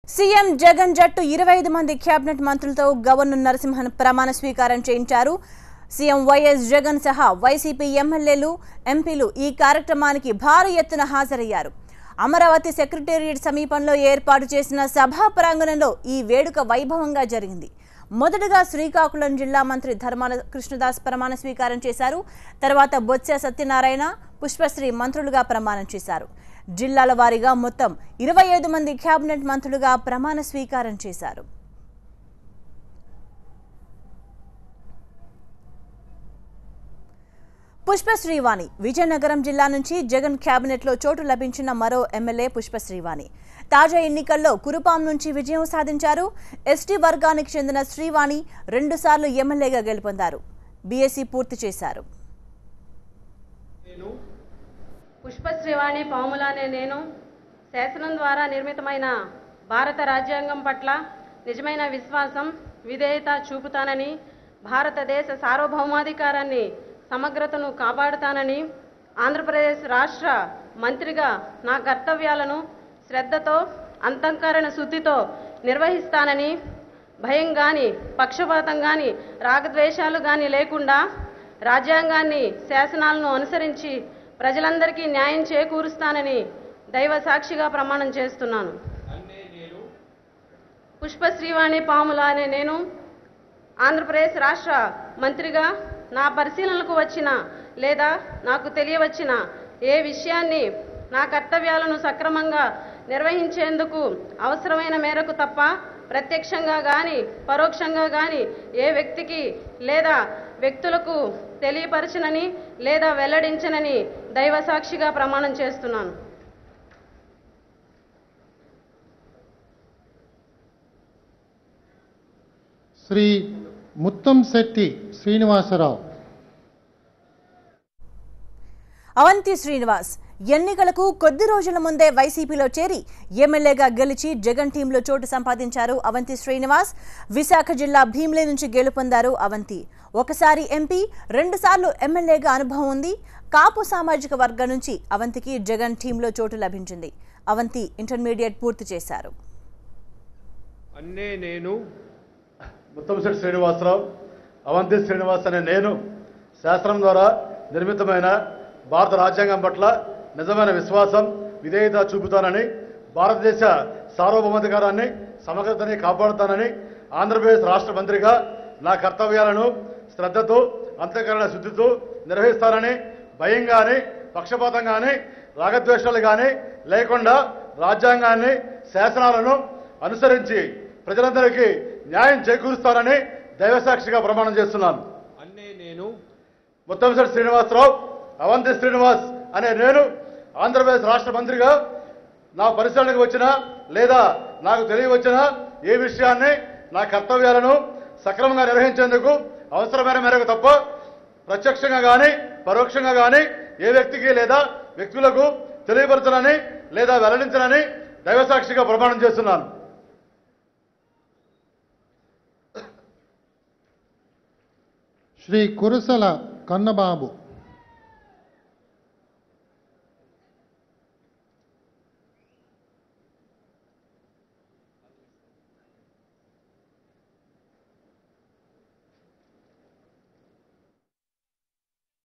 YCP MPs and MLAs participate in swearing in ceremony - TV9
YCP MPs and MLAs participate in swearing in ceremony Watch LIVE